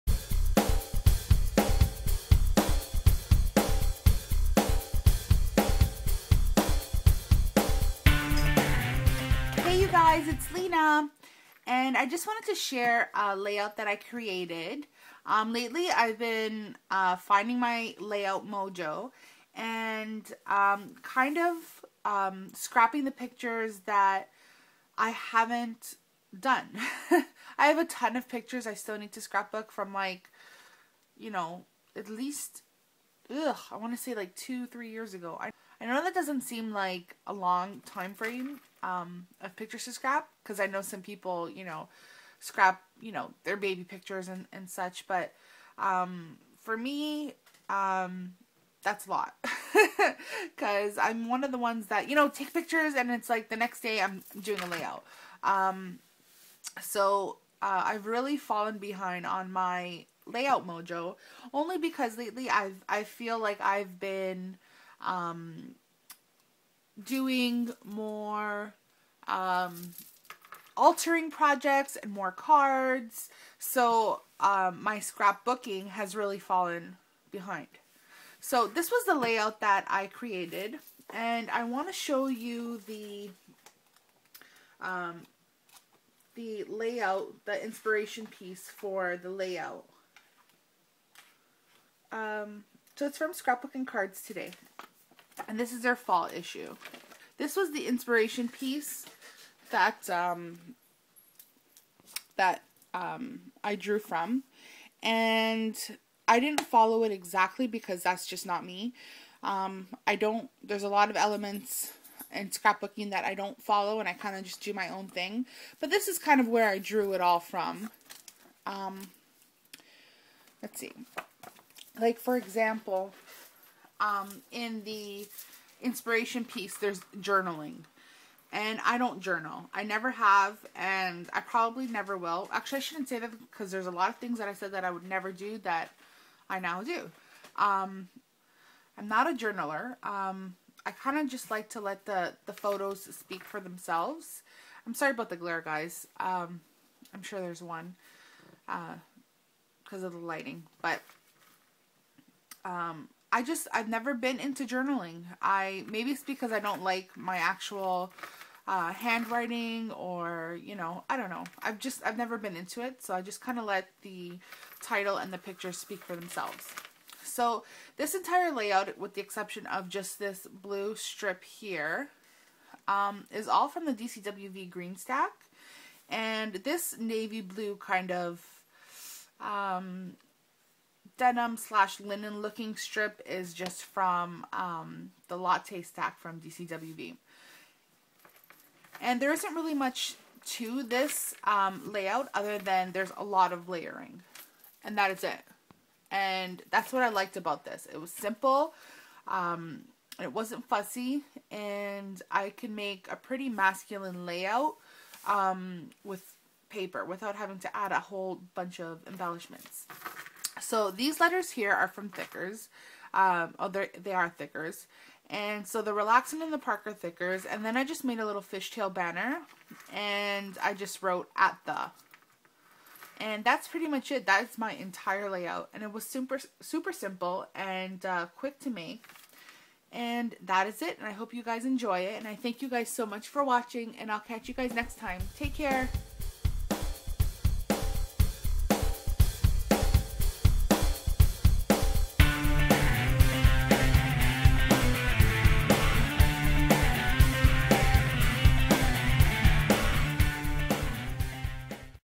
12x12 Layout- Relaxin' At The Park
Hey guys! Here's a layout I created with a few pictures from the summer! It's more of a different layout with a darker, more masculine set of colours. Hope you enjoy!

xo Lina